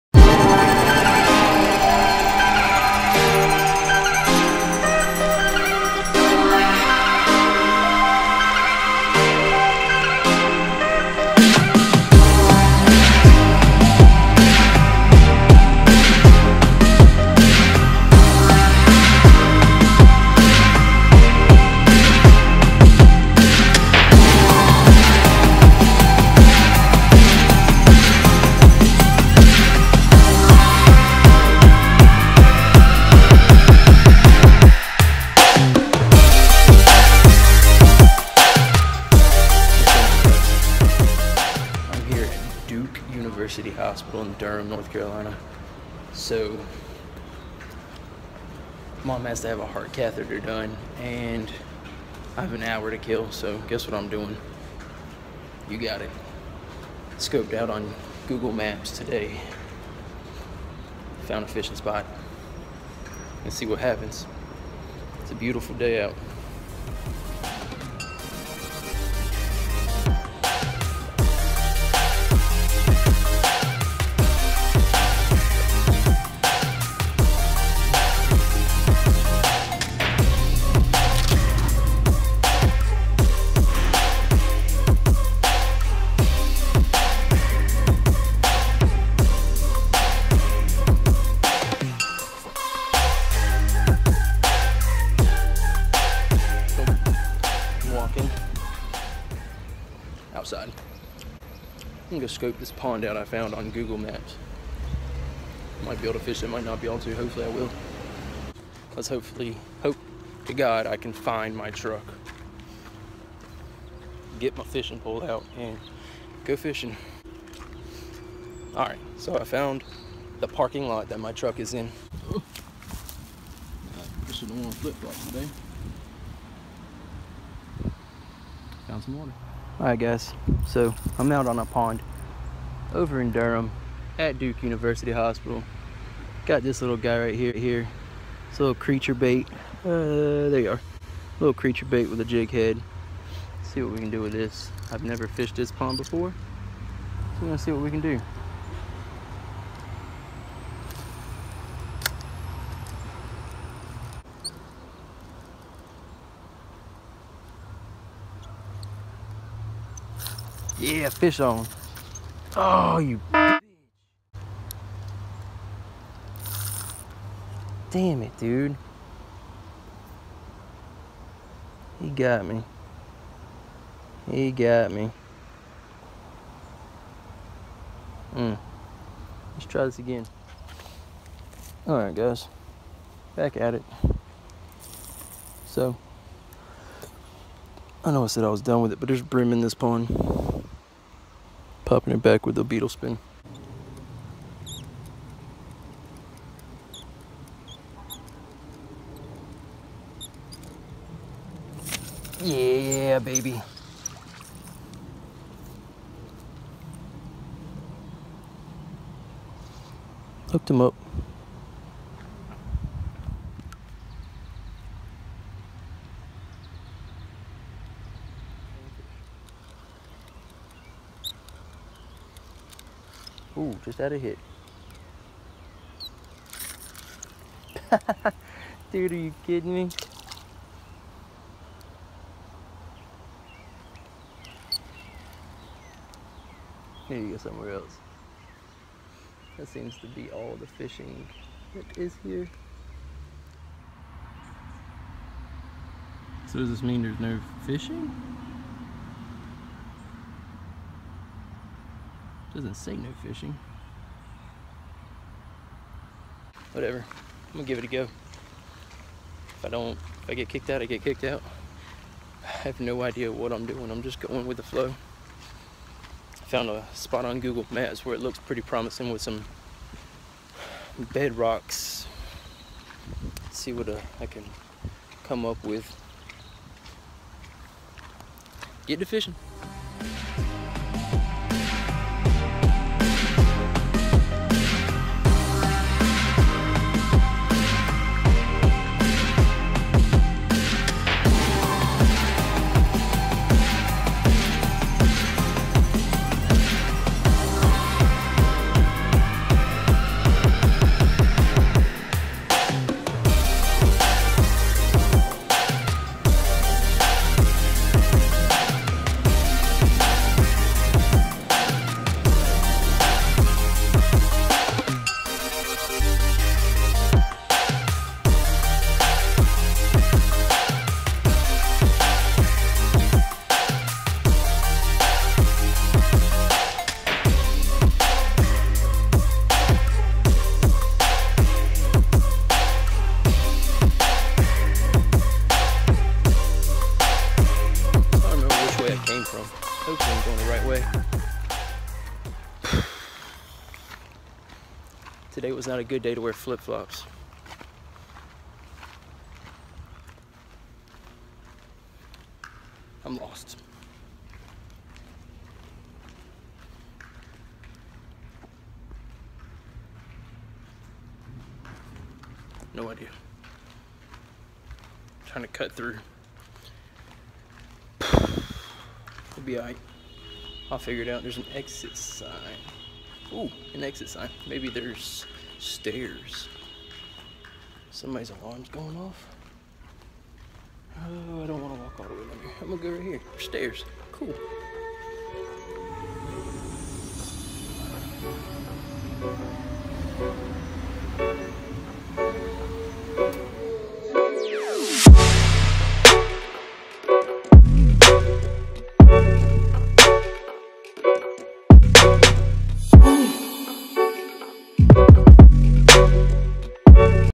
ALMOST CAUGHT FISHING!!! Fishing at Duke University Hospital!!
MOM had a heart cath done today. So naturally I decided to fish!!

SIDE NOTE:
MOM had a heart transplant on September 4, 2017. She is doing great and living on her own now!! Thanks to all the support that friends and family gave her!!

Back to fishing, same old set up I've used in the past.

If you're in the lookout for a waterproof bag, cop you one of these bad boys!!

I use one every time I fish and I love mine.

SNAP:
zachcrowe

LURES USED:

ARKIE CRAWLIN JIG:

Thats really the only one I used in this video.

beside the beetle spin.

thanks for watchin guys and gals!!

remember

GET

TO

fishing.